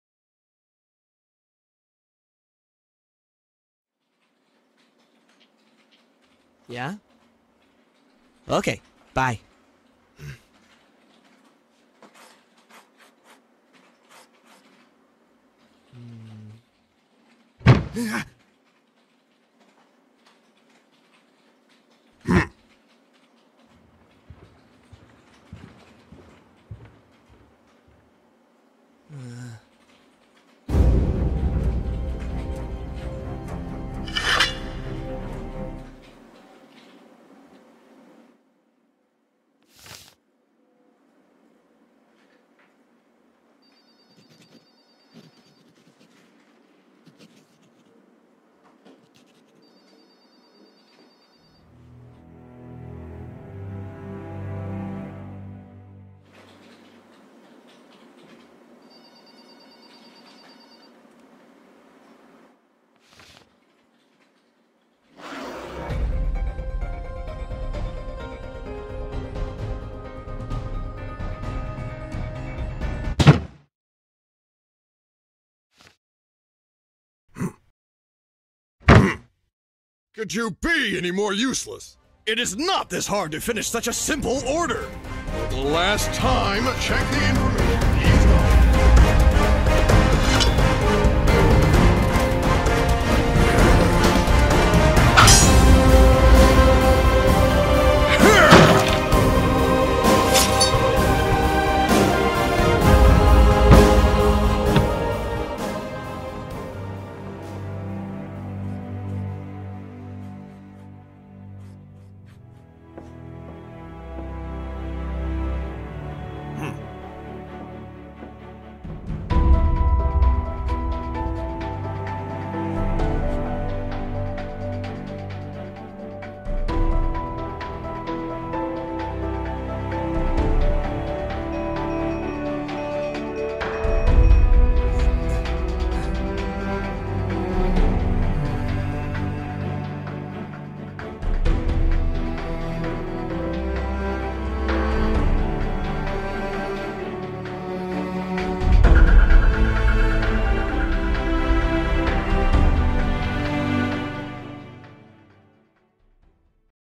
BOSS-Original Animated Short
This story was motivated by the frustration i felt working on a job in which creativity was out of place. And most of the conversations with coworkers had this phrase on them: it's almost impossible to get to work in what you love". But i kept working on my portfolio and now i'm working as 2d animator. After all of what happened, i know that you will always reach your goals as long as you keep working to reach them.